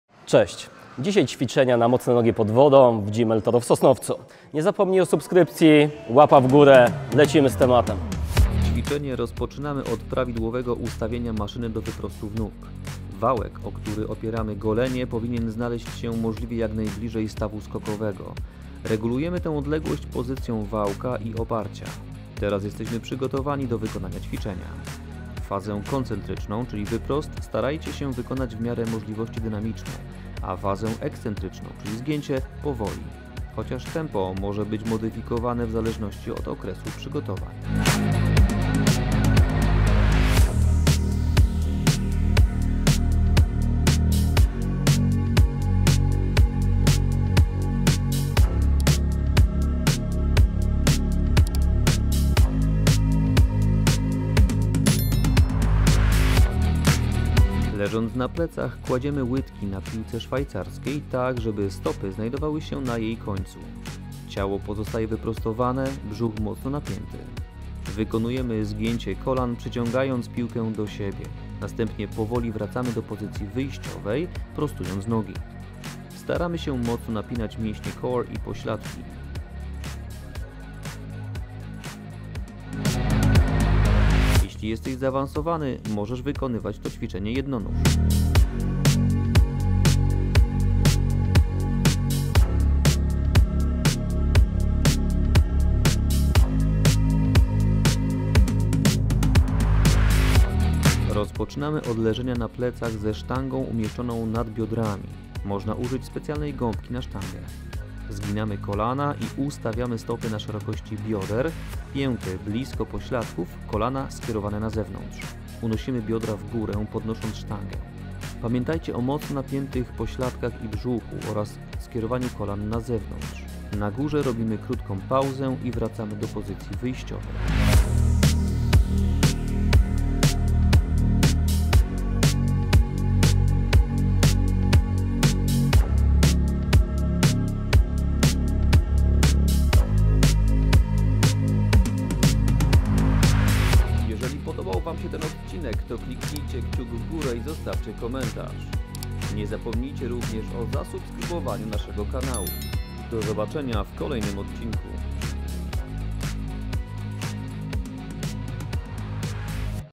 3 najlepsze ćwiczenia na siłowni na mocne nogi pod wodą - Lekcja 17 sezon 3
Chcesz wzmocnić pracę nóg w pływaniu? Koniecznie oglądaj ten odcinek! Przedstawimy najlepsze 3 ćwiczenia!